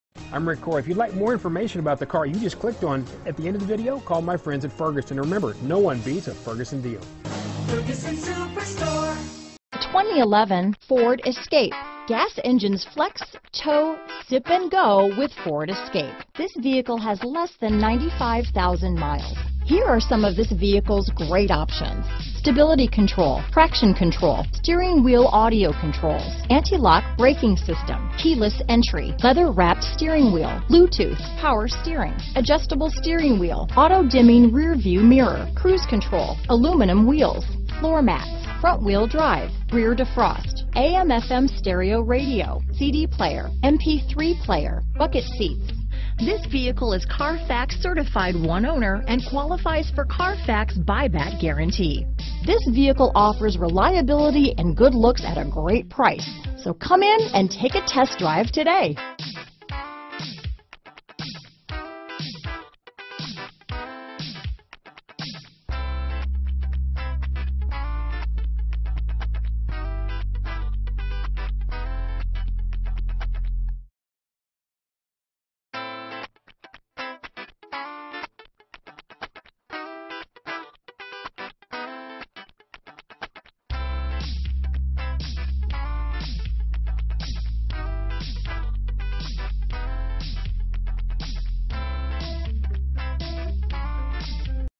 2011 Ford Escape Tulsa, Broken Arrow, Owasso, Bixby, Green Country, OK G70722B
2011 Ford Escape

Ferguson Superstore
1601 N Elm Place

New Price!2011 Ford Escape Limited 4D Sport Utility CARFAX One-Owner. Priced below KBB Fair Purchase Price!Escape Limited, Duratec 3.0L V6 Flex Fuel, FWD.Awards:* 2011 KBB.com Brand Image Awards